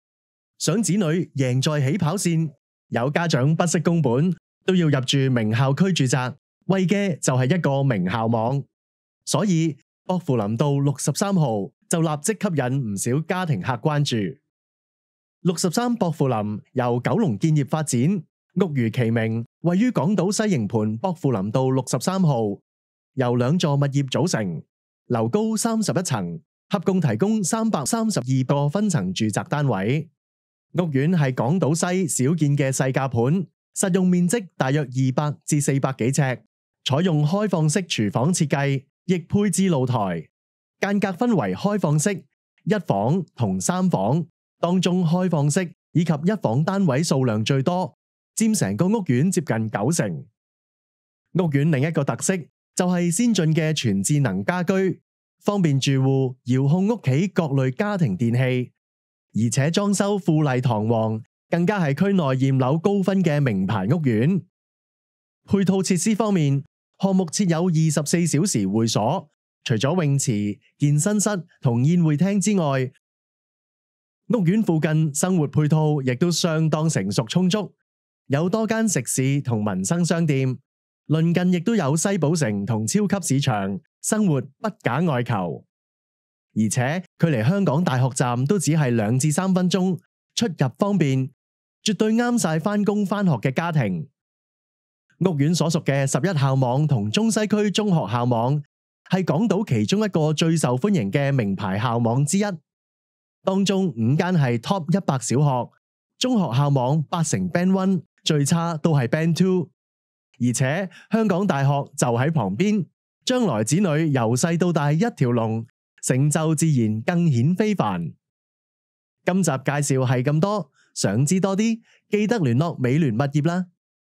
【63 POKFULAM】豪華物業一流校網
想子女贏在起跑線
有家長不惜工本
都要入住名校區住宅
為了就是一個名校網
所以薄扶林道63號
就立即吸引不少家庭客關注
63薄扶林
由九龍建業發展
屋如期名
位於港島西營盤薄扶林道63號
由兩座物業組成
樓高31層
合共提供332個分層住宅單位
屋苑是港島西少見的細價盤
實用面積大約200至400幾呎
採用開放式廚房設計
亦配置露台
間隔分為開放式
一房同三房
當中開放式
以及一房單位數量最多
佔整個屋苑近九成
屋苑另一個特色
就是先進的全智能家居
方便住戶遙控屋企各類家庭電器
而且裝修富麗堂皇
更加是區內驗樓高分的名牌屋苑
配套設施方面
項目設有24小時會所
除了泳池
健身室及宴會廳之外
屋苑附近生活配套亦都相當成熟充足
有多間食肆同民生商店
鄰近亦都有西寶城同超級市場
生活不假外求
而且距離香港大學站都只是兩至三分鐘
出入方便
絕對適合返工返學的家庭
屋苑所屬的11校網同中西區中學校網
是港島其中一個最受歡迎的名牌校網之一
當中5間是top 100小學
中學校網班成band1
最差都是band2
而且
香港大學就喺旁邊
將來仔女由細到大一條龍
成就自然更顯非凡
今集屋苑介紹到此為止
如果想更多
記得聯絡美聯物業

想了解更多屋苑資訊，立即按此向在線客服查詢啦！